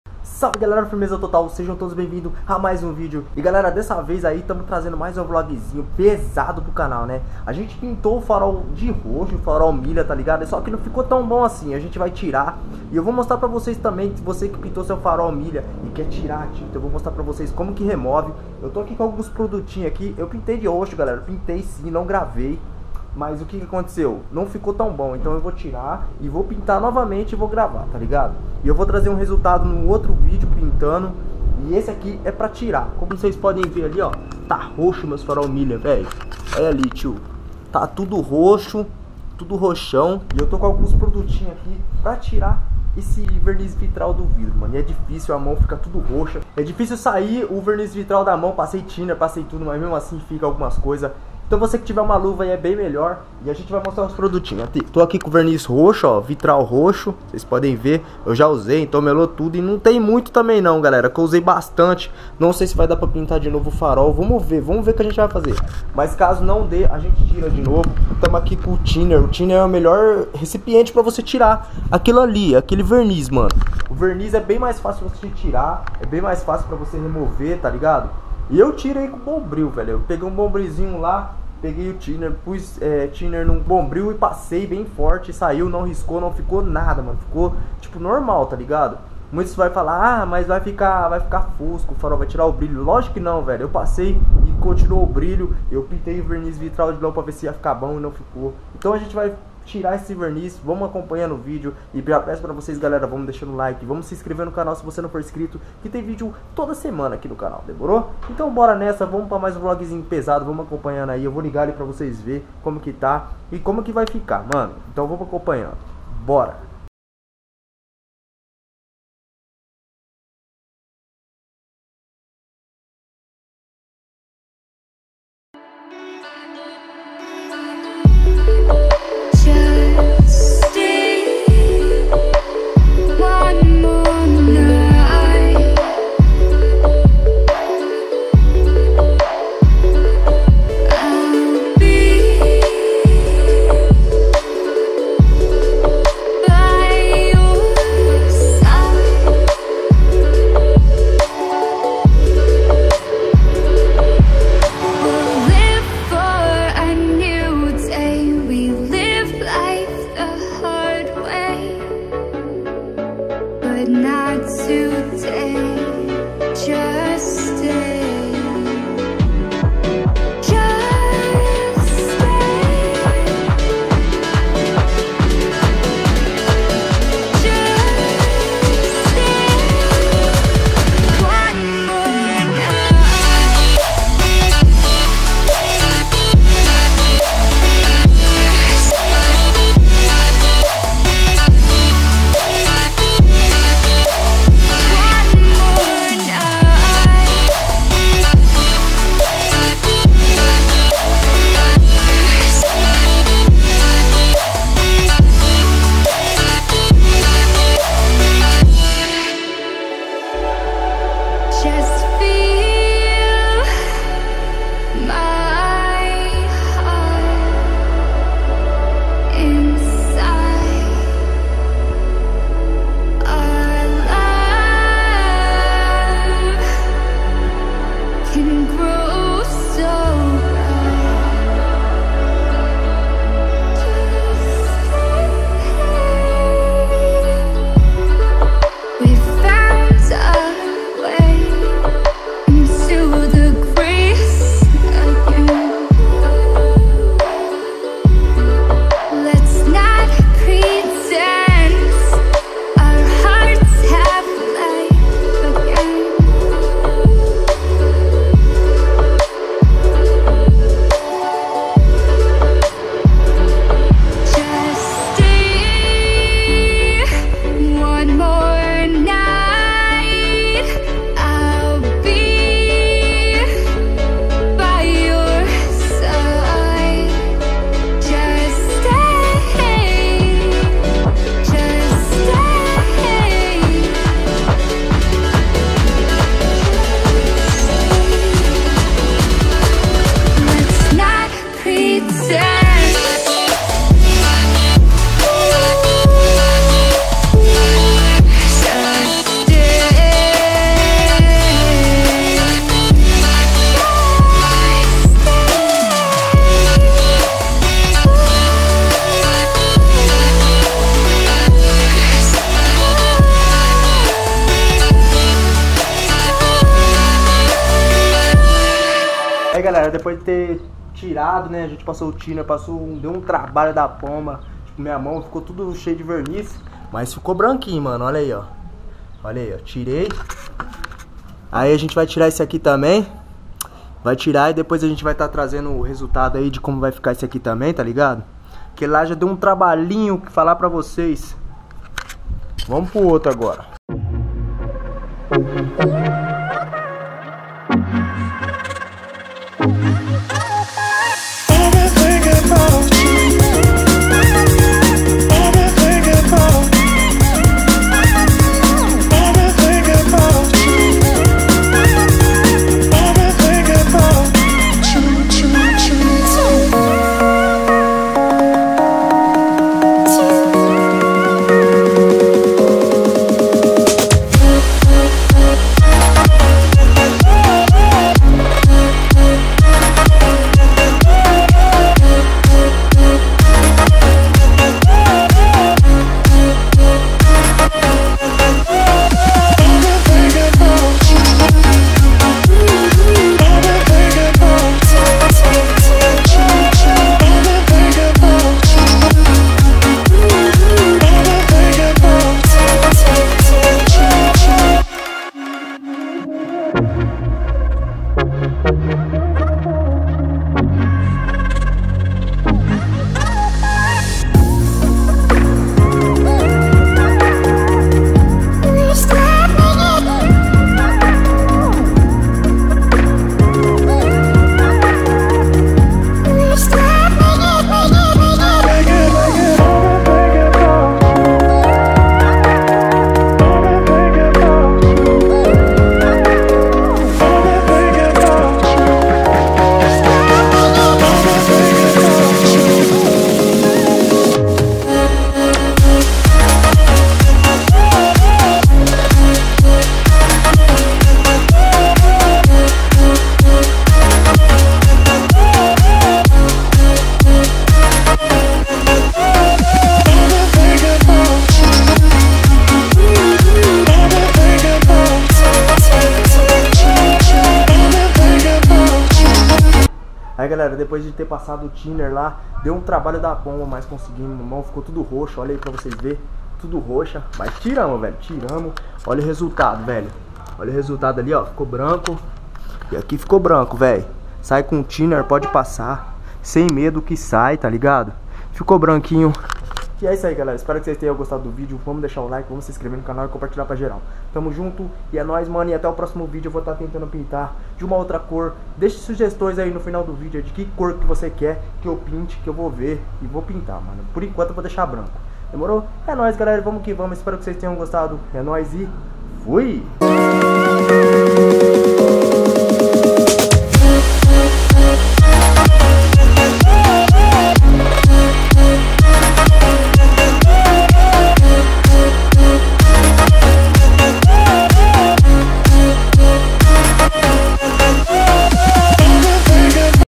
Como Deixar Seu Farol Igual Xenon
Mais um video na area galera espero que curtam , bora deixar os like e compartilhar , espero que curtam , pra quem passo verniz vitral no farol e nao sabe como rancar 100% ta  ai uma otimaa dicaa pra voce tmj

► Carro : VOYAGE QUADRADO ANO 85